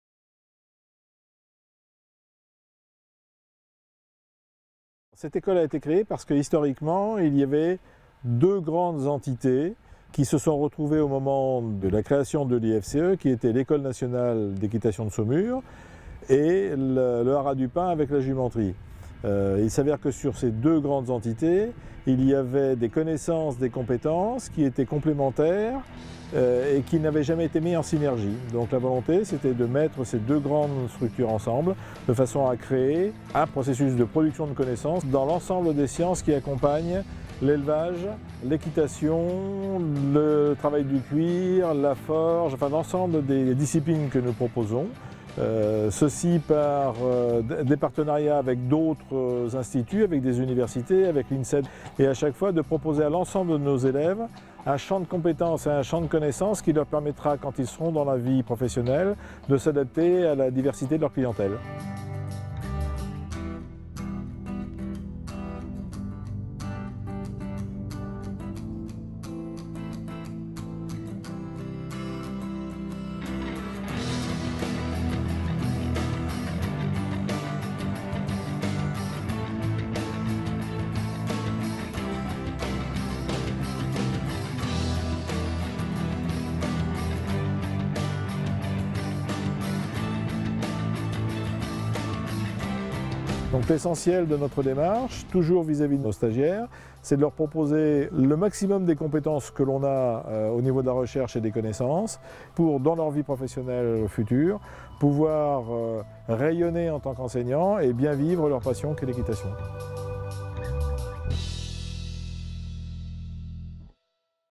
Ecole Supérieure du Cheval et de l'Equitation (Teaser)
L’école supérieure du cheval et de l’équitation, issue de la mise en synergie de deux écoles renommées, l’École nationale d’équitation à Saumur et l’École nationale professionnelle des Haras au Pin, propose un large panel de formations d’excellence dans les domaines de l‘élevage, de la reproduction, de l’équitation, de la sellerie, de la maréchalerie et de l‘attelage.

Venez découvrir les formations qui y sont proposées !